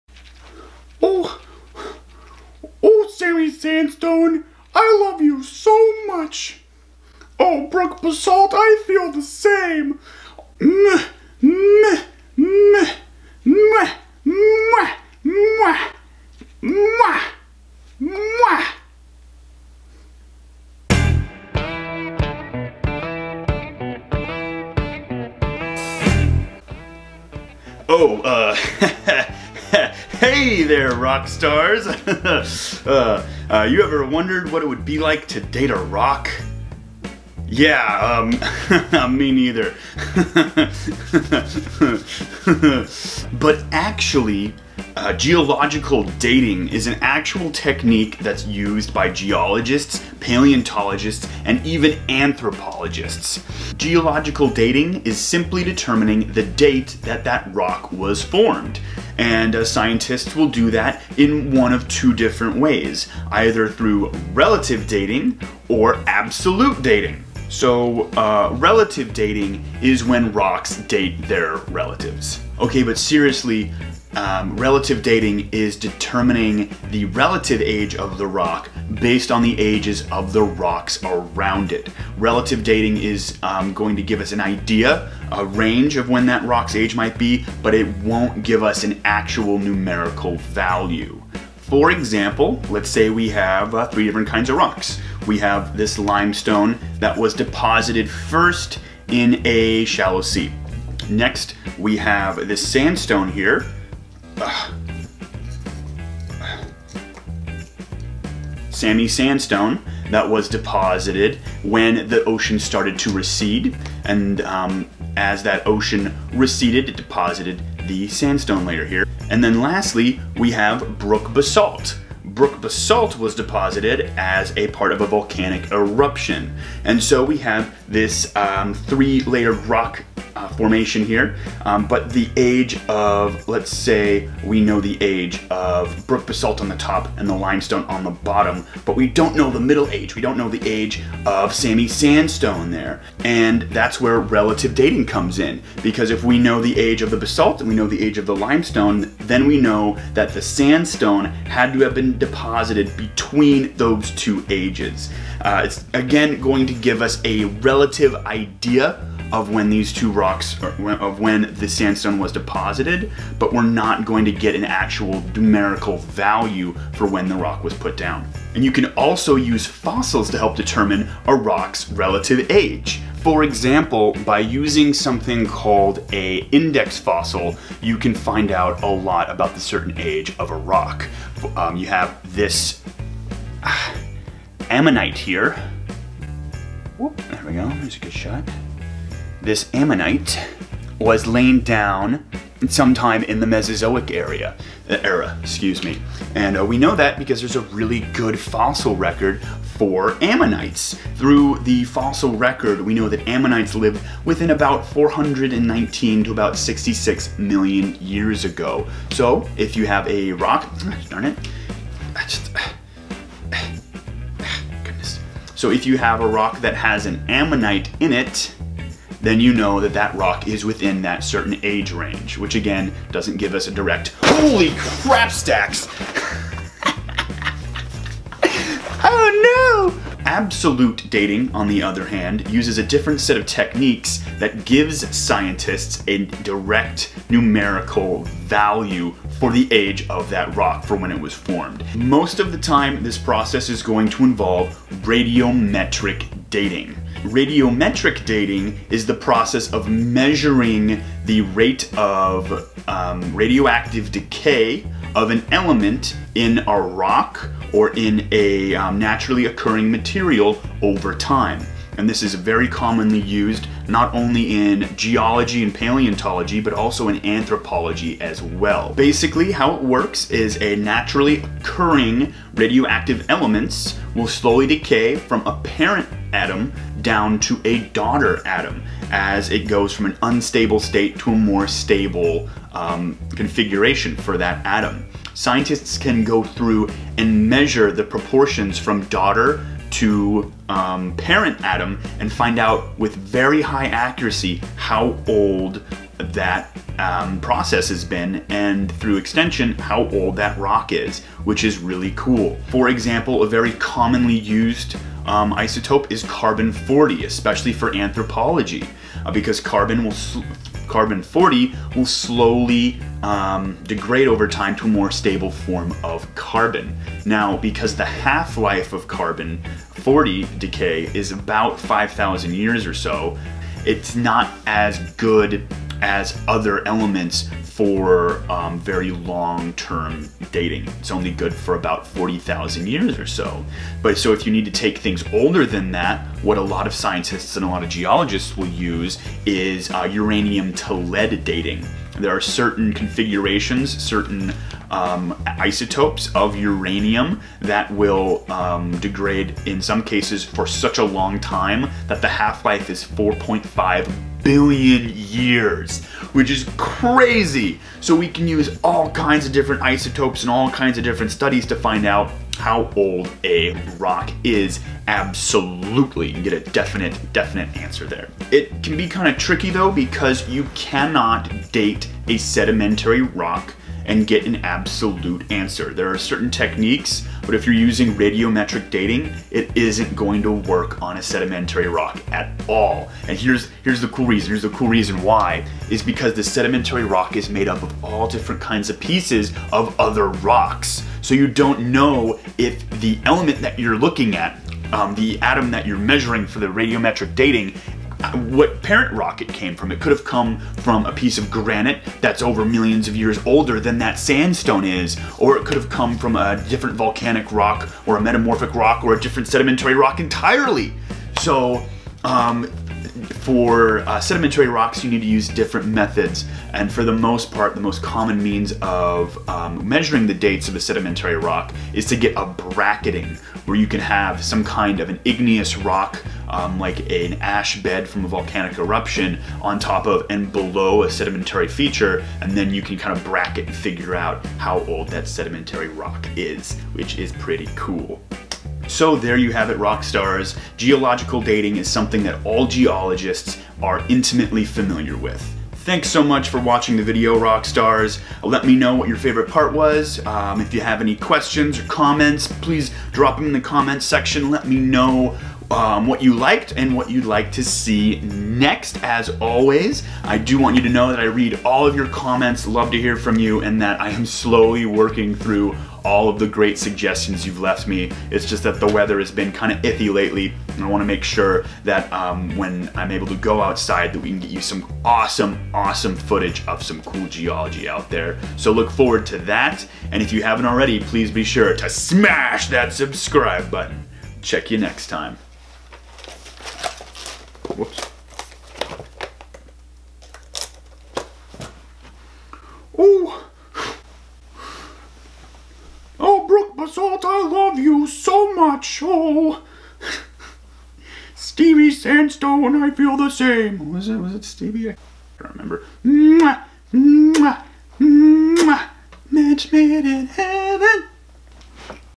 Dating Rocks?!
A little late for valentines day... but nonetheless I wanted to see how geologists date rocks! Thats right, the act of determining a rocks age, or dating, is a very important process that is used by a host of scientific disciplines!